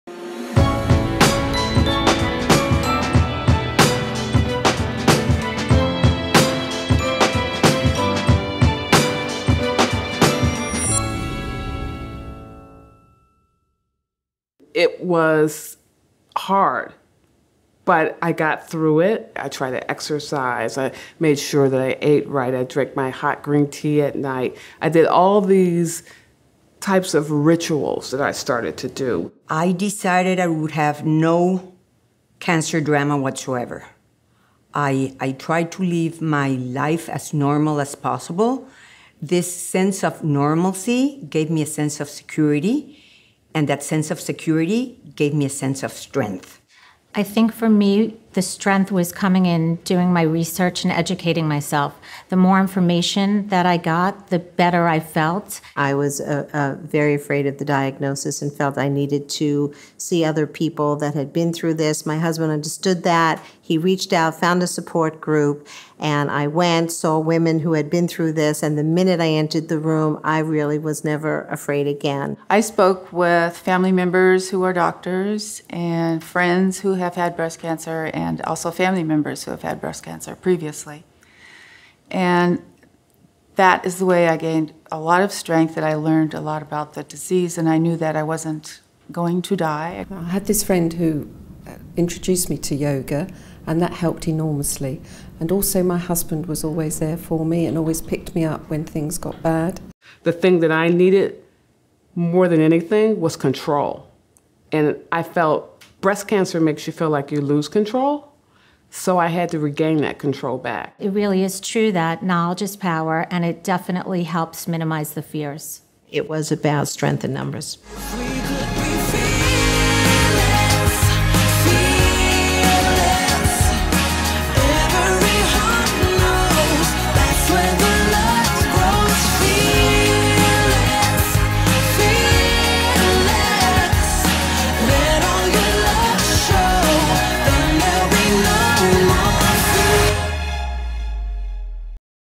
BCA Campaign Profiles of Strength: Taking Action
"Profiles of Strength" is a new video series from The Breast Cancer Awareness Campaign that spotlights the inspiring strength of employees at The Estée Lauder Companies whose lives have been personally touched by breast cancer. Watch as six of our inspiring colleagues share how they take action against breast cancer — from participating in daily rituals to connecting with their support systems. Breast cancer touches all of us. With support, we're stronger together.